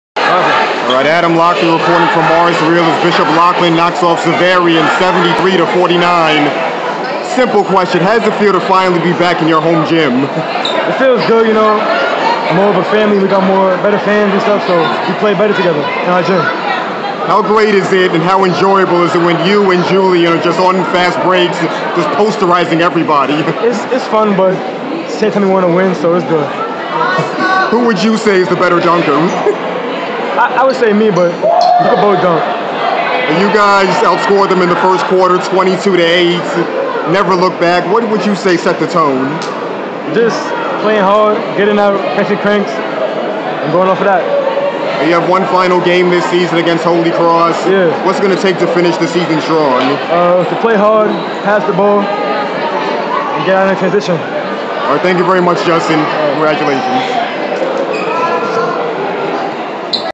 Justin Champangie Mars Reel Interview
My interview with Bishop Loughlin Junior Justin Champangie following their 73-49 victory over Xaverian on Saturday afternoon